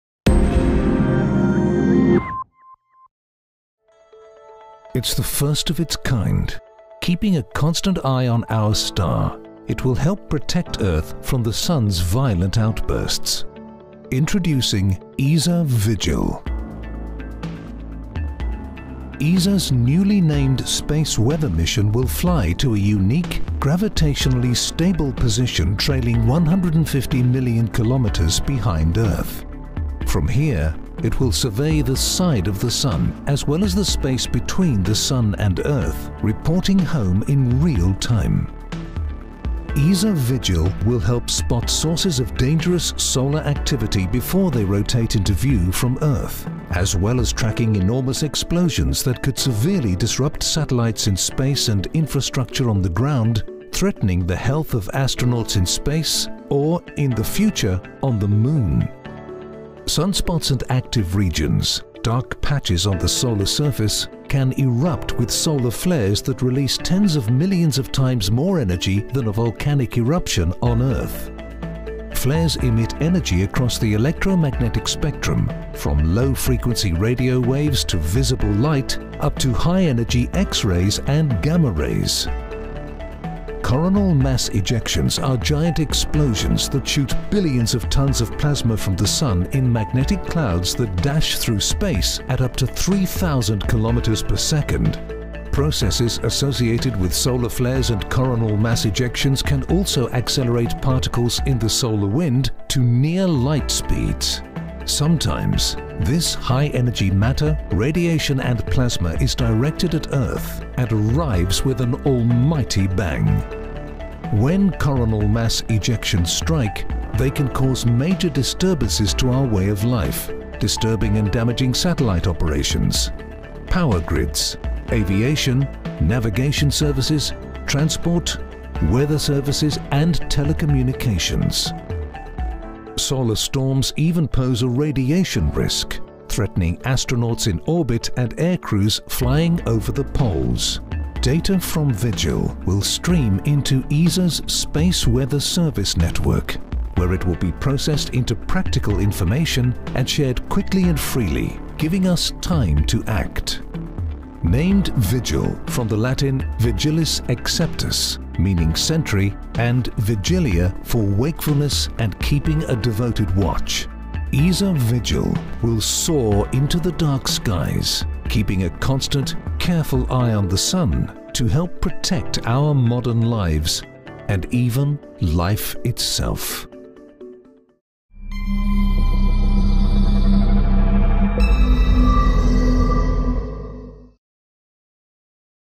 New Vigil sun observatory will track solar explosions & more from space
ESA's observatory Vigil will 'spot sources of dangerous solar activity' on the sun before it rotates into Earth view. Learn more about the mission in this overview.

Credit: ESA